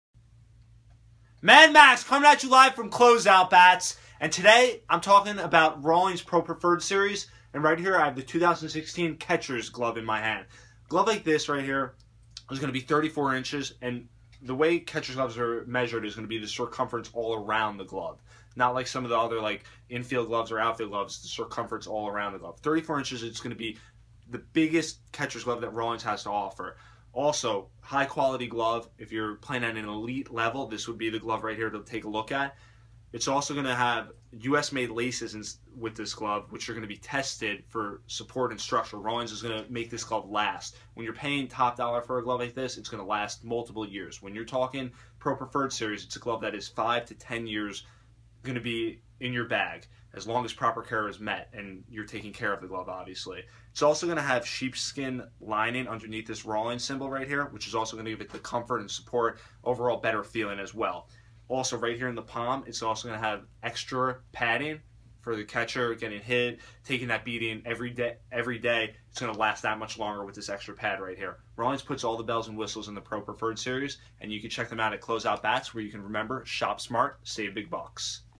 2016 Rawlings PROSCM43BC 34IN Closeoutbats.com
Mad Max talks about the New Rawlings Pro Preferred model Catcher's Mitt. This 34inch glove is for the elite catcher playing Highschool, College or even Pro level.